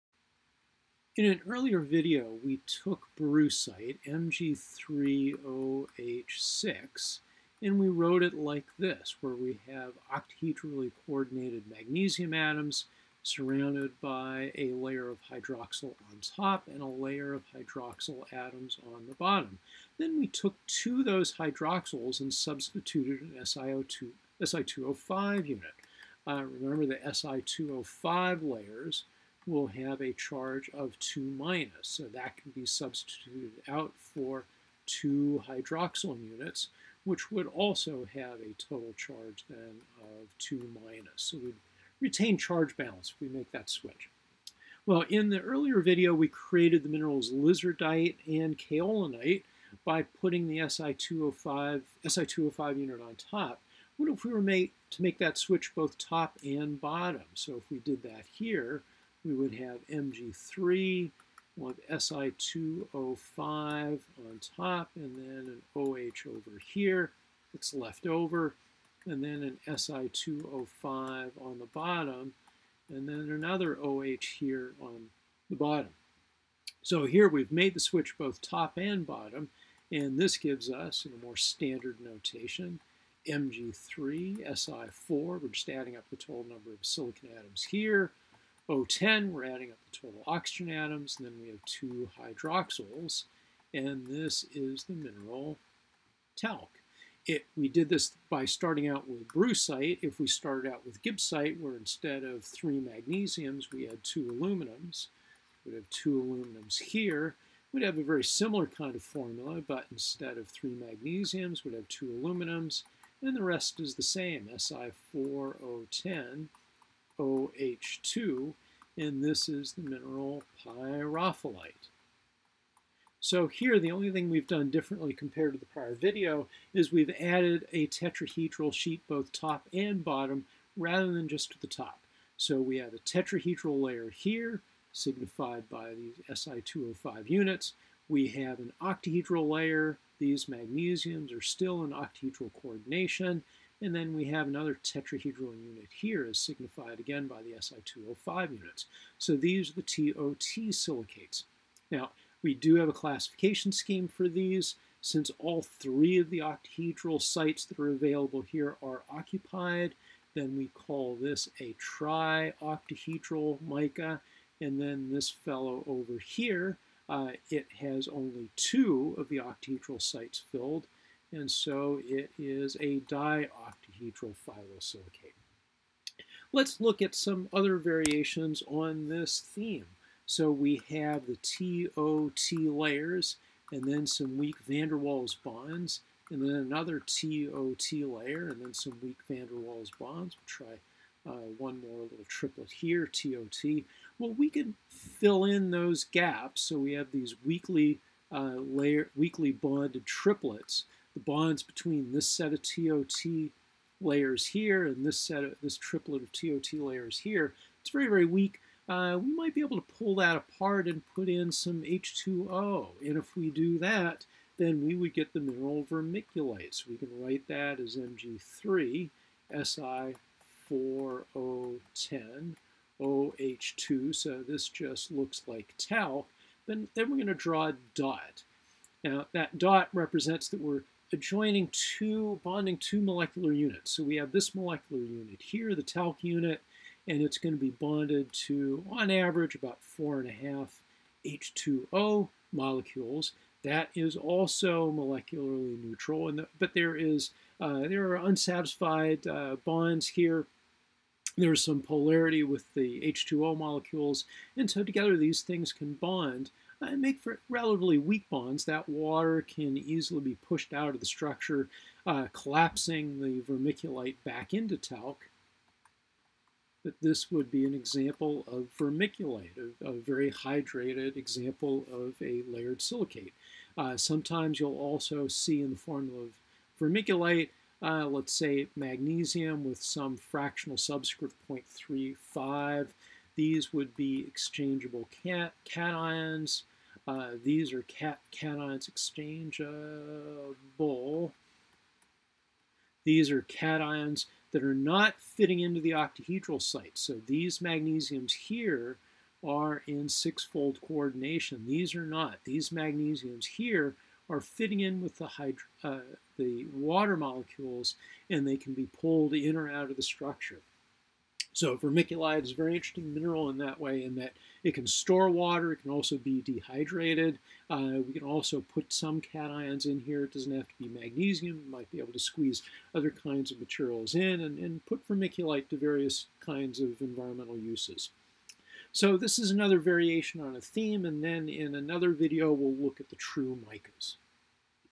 Phyllosilicates 3: t-o-t structures (talc, pyrophyllite, vermiculite)
Introduces the structure and chemistry of the t-o-t phyllosilicates, using talc, pyrophyllite and vermiculite as examples (by KeithPutirka)